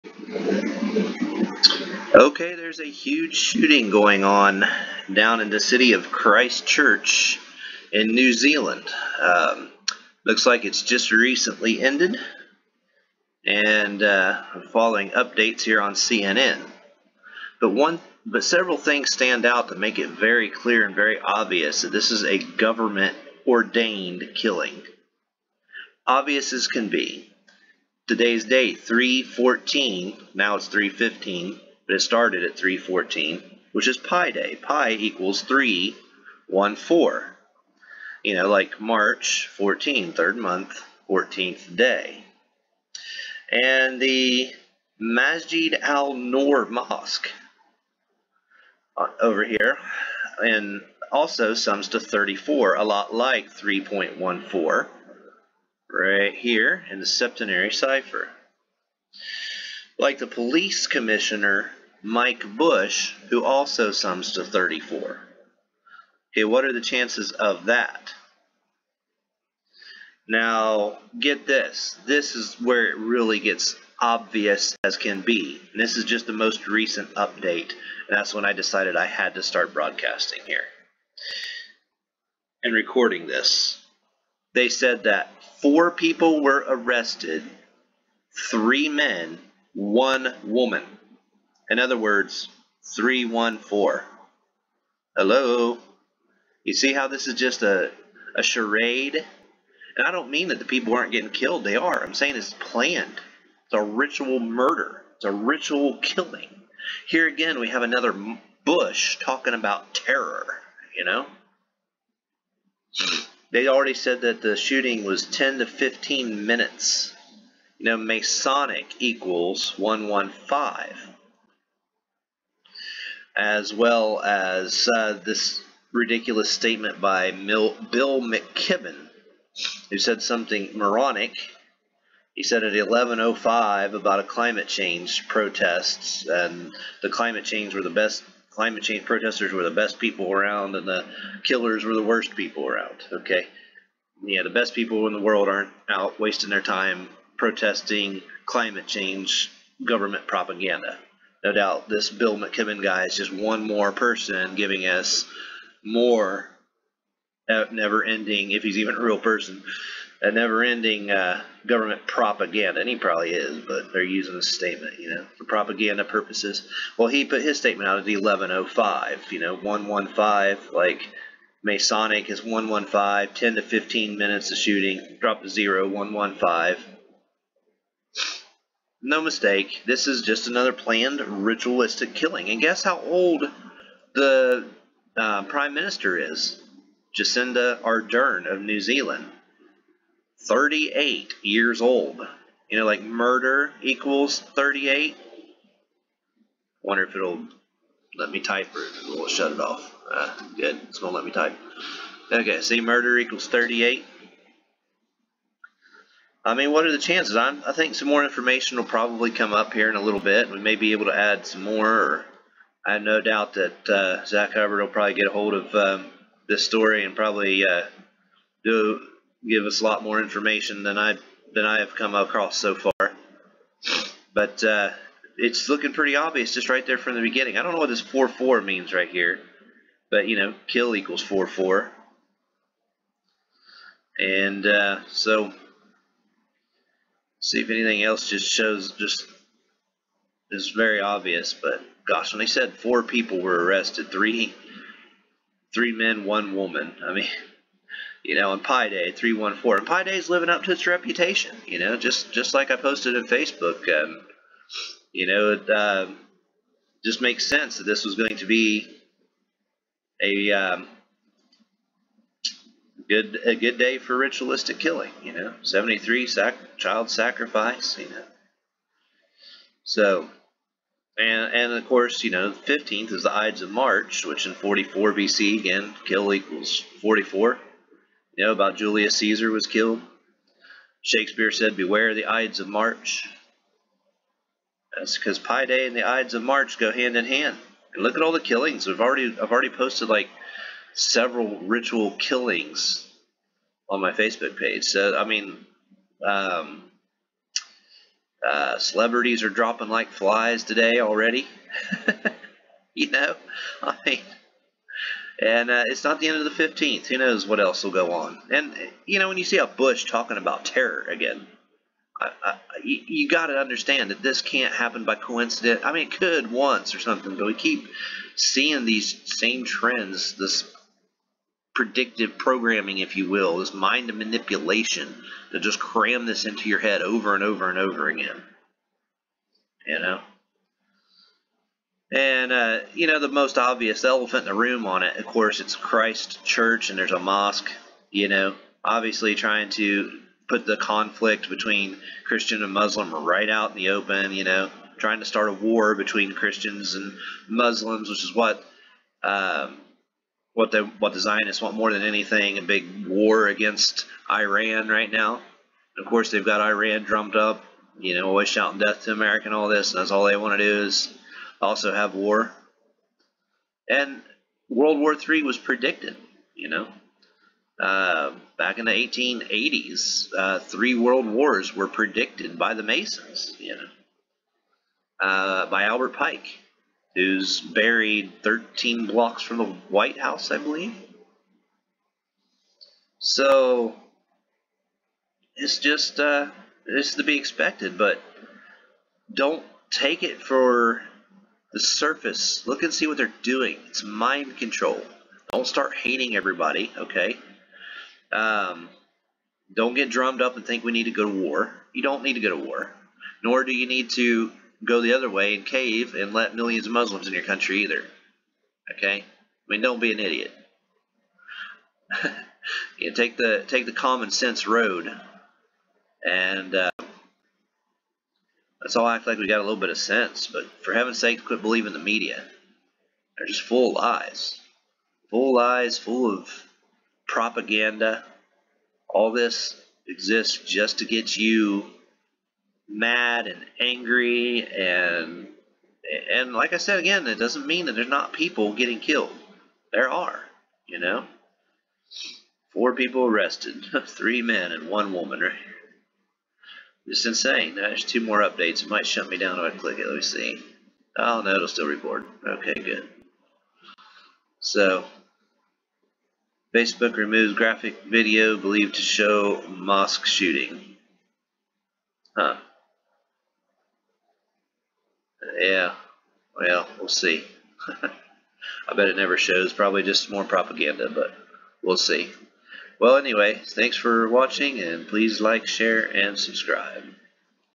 4 Arrested, 1 Woman, 3 Men on Pi Day In Mosque Shooting in New Zealand
This is another OVERTLY OBVIOUS, planned, ritual killing by the elitists who run this world. They can't make it any more obvious!   ** Latest update** "Attacker posted 87 page manifesto." The word "Muslim" = 87 in EO.